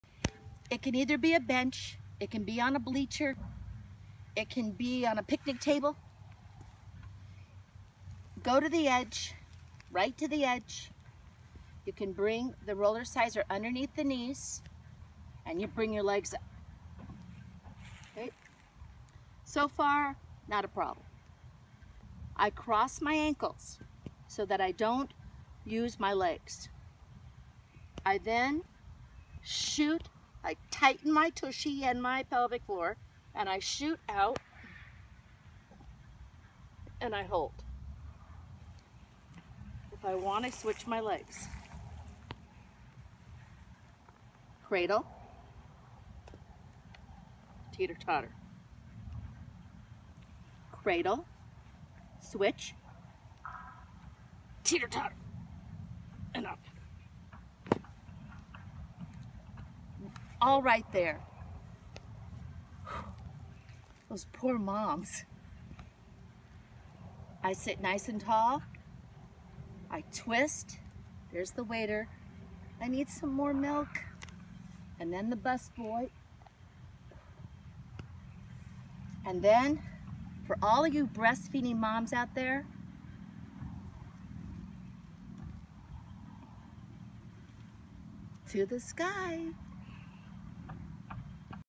Strollercize® New Mom Ab Exercise on Park Bench By Elizabeth Trindade
Author of Strollercize®, Elizabeth Trindade shows a move that is used in her Certified Instructors classes. She calls this Cradles. It uses her invention The RollerSizeR which comes with Registration. It makes working out easier and targeting the tummy of a new Mom's belly faster!